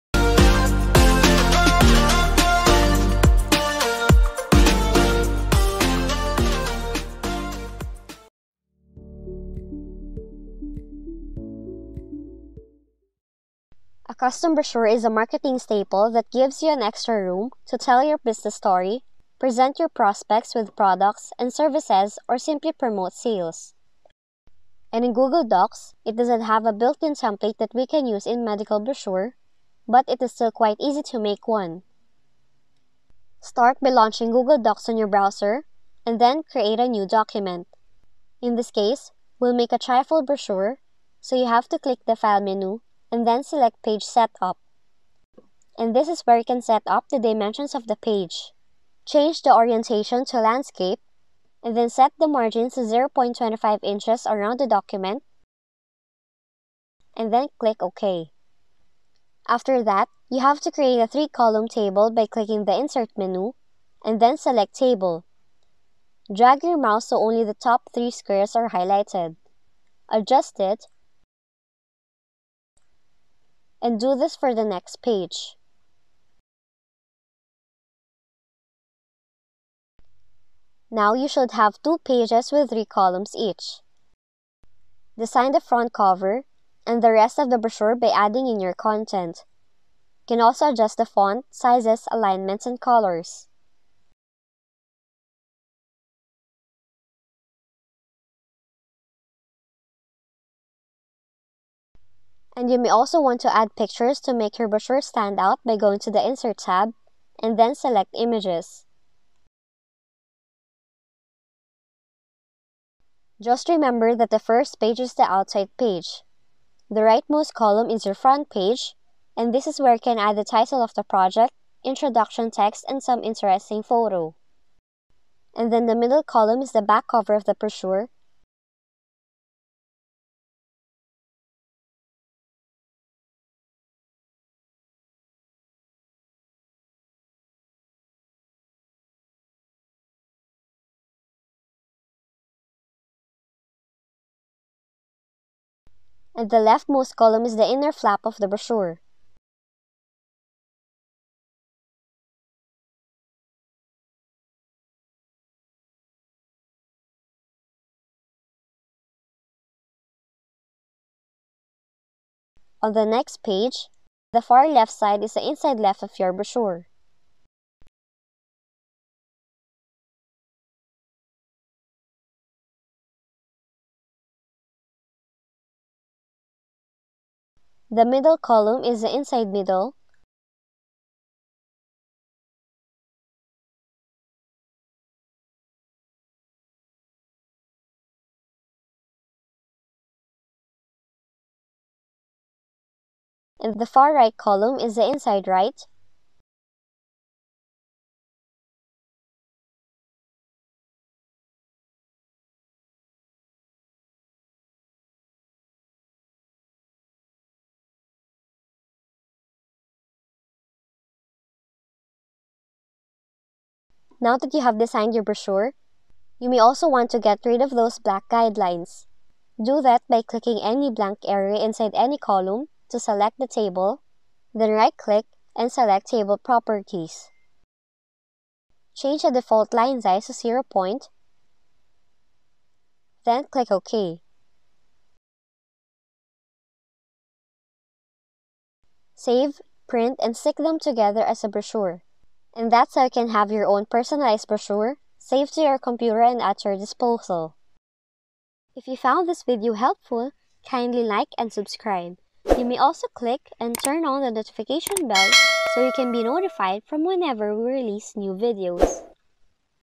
[HOW-TO] Create a Printable (Trifold) MEDICAL BROCHURE in Google Docs
--- Need to create a medical brochure, but not sure where to start?  This easy-to-follow tutorial will show you exactly how you can create a medical (or any other type) of brochure easily!  You'll first want to compile what information you want to include, and which page it will go on.  Then add it to the template that I show you how to create! ---